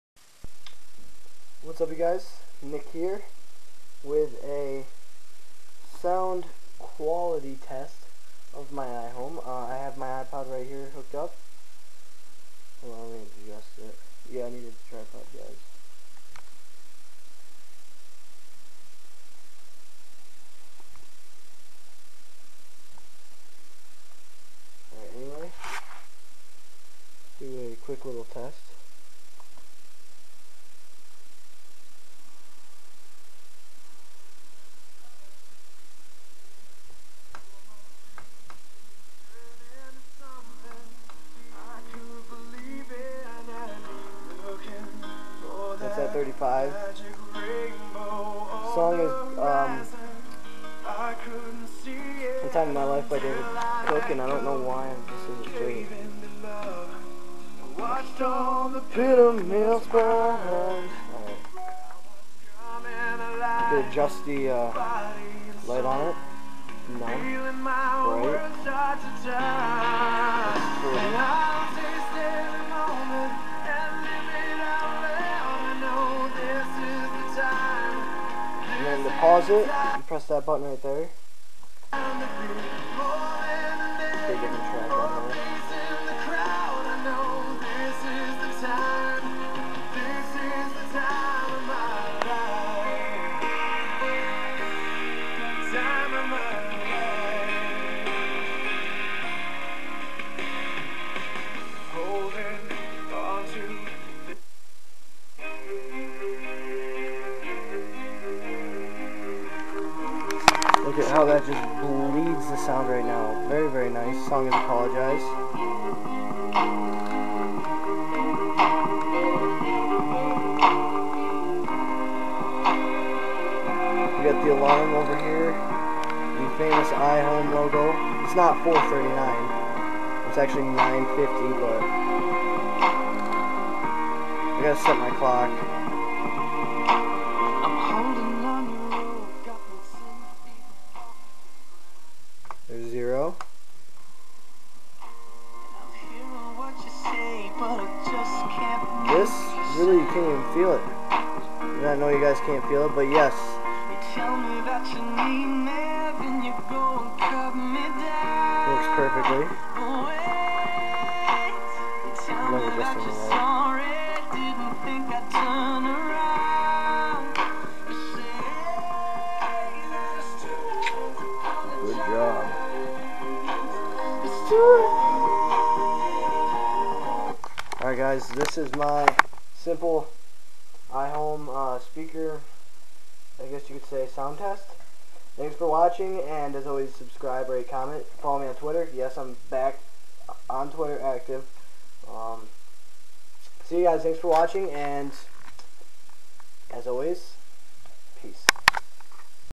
iHome Sound Test
This is a video of my sound test for my iHome which I have had for a while so. This will be just a quick video so I hope you enjoy this video.